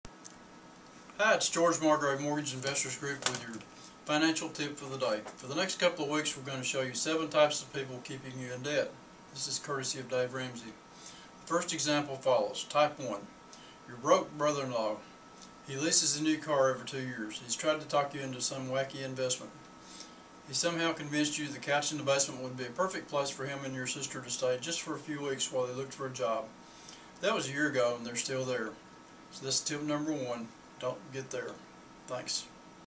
Financial Tip 092313
For the next couple of weeks we are going to show you 7 types of people keeping you in Debt. (courtesy of Dave Ramsey.)  The examples follow.
Type one: Your broke brother-in-law
He leases a new car every two years.  He has tried to talk you into wacky investment schemes on a regular basis.  He somehow convinced you that your couch in the basement would be a perfect place for him and your sister to stay "just for a few weeks while he looked for a job."  That was a year ago and they are still there.